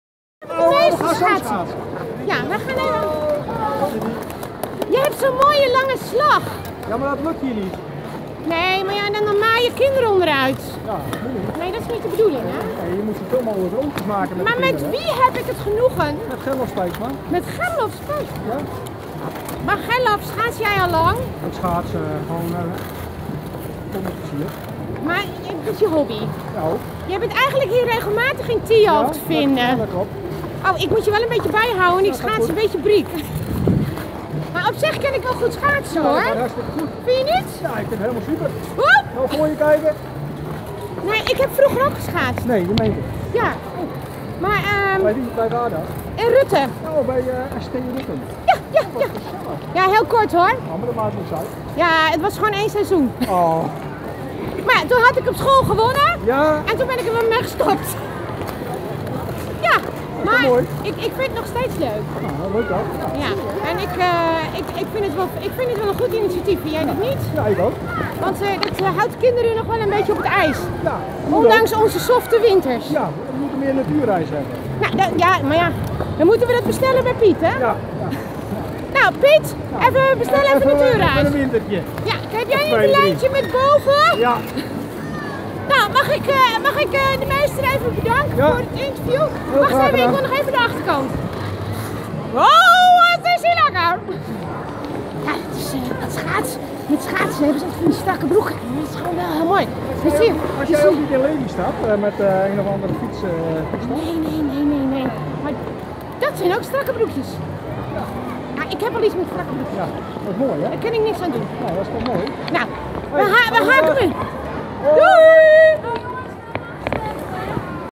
Rondje met de Meester ..poeh net Snelle Jelle
Brechje bij Schaatsen voor Water ,Thialf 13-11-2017 middagploeg